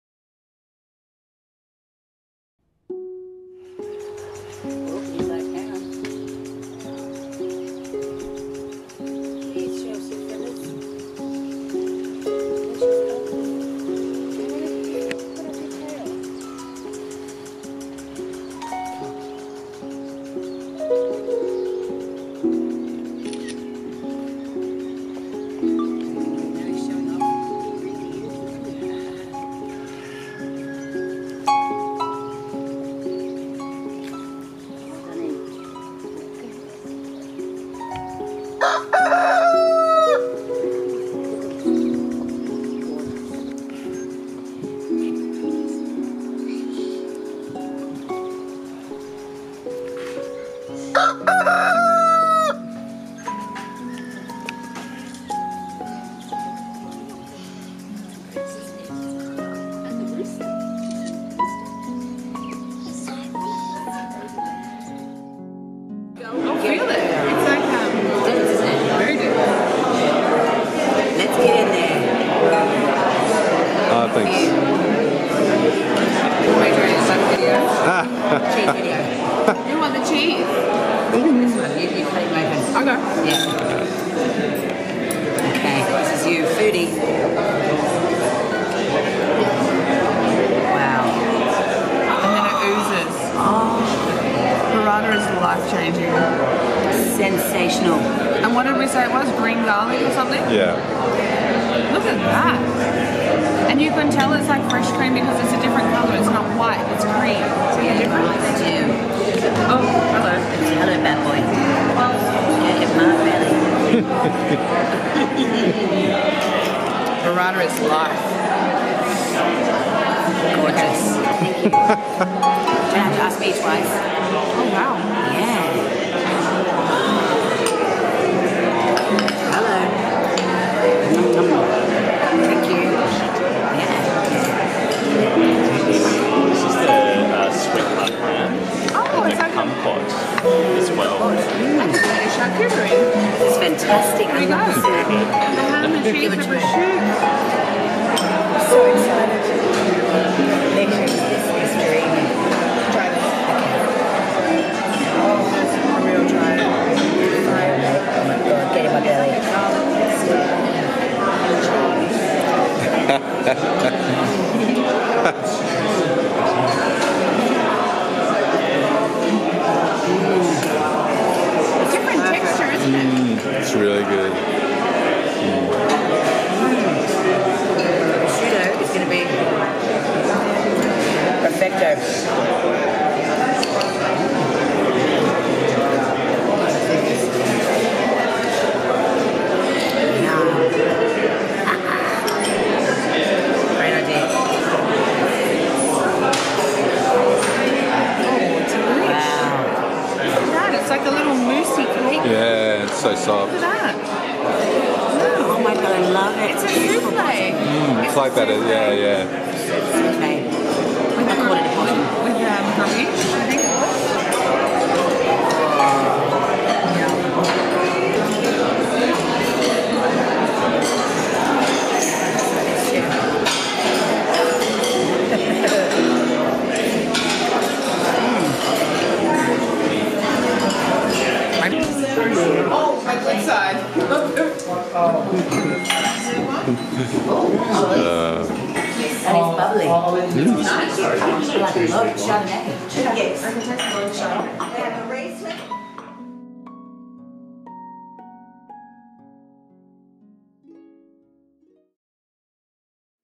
Tasmania Gourmet Food Tour - luxury small tours
A day in the life of Tasmania Gourmet Food Tours !
Where we wine and dine you at some the finest establishments the island has to offer
Fresh seafood, local and seasonal specialists
We are small group luxury tours
Complimentary photo/video postcards are included !
Pick up and drop off included
Tours depart daily out of Hobart, the capital of Tasmania
Come and relax with us today the way the locals do